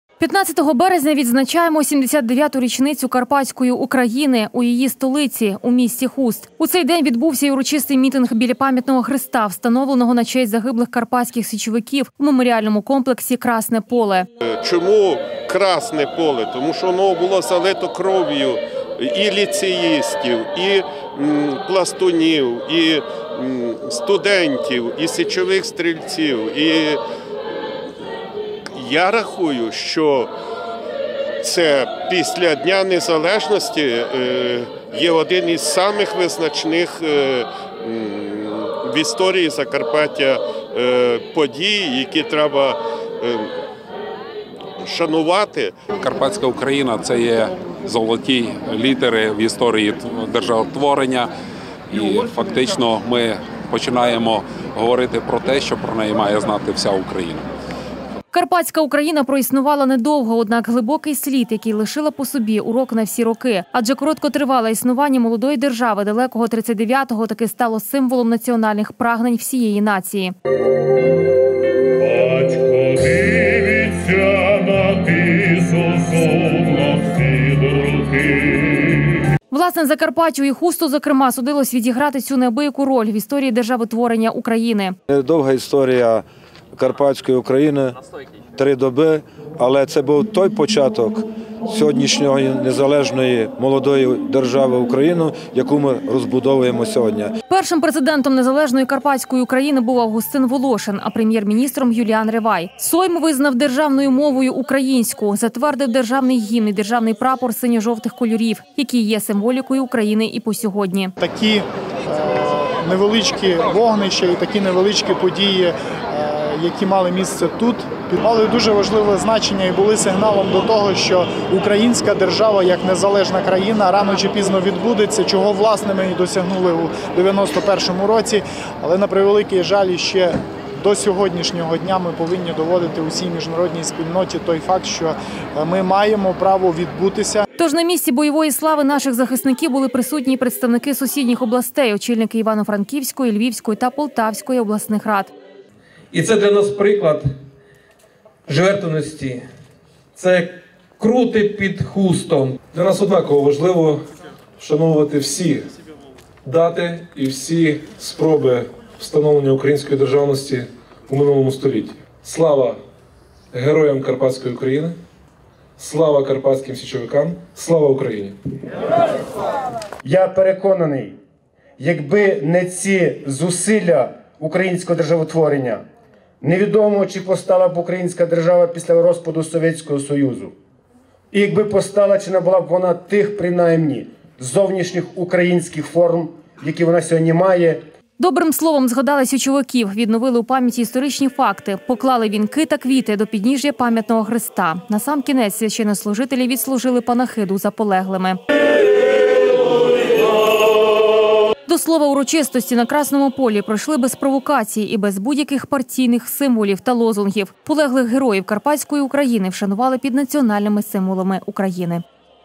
Урочистості до 79-річниці Карпатської України
15-го березня відбувся урочистий мітинг біля Пам’ятного Хреста, встановленого на честь загиблих Карпатських січовиків, у меморіальному комплексі Красне Поле.Карпатська Україна проіснувала недовго. Однак глибокий слід, який лишила по собі- урок на всі роки. Адже короткотривале існування молодої держави далекого 1939-го таки стало символом національних прагнень всієї нації. Власне, Закарпаттю, і Хусту зокрема, судилось відіграти цю неабияку роль в історії державотворення України. Першим Президентом незалежної Карпатської України був Августин Волошин, а прем’єр – міністром - Юліан Ревай. Сойм визнав державною мовою – українську, затвердив державний гімн й державний прапор синьо-жовтих кольорів, які є символікою України й по сьогодні. Тож, на місці бойової слави наших захисників були присутні й представники з сусідніх областей – очільники Івано-Франківської, Львівської та Полтавської обласних рад.Добрим словом згадали січовиків, відновили у пам’яті історичні факти , поклали вінки та квіти до підніжжя Пам’ятного Хреста. Насамкінець, священнослужителі відслужили панахиду за полеглими. До слова, урочистості на Красному Полі пройшли без провокацій і без будь-яких партійних символів та лозунгів. Полеглих героїв Карпатської України вшанували під національними символами України.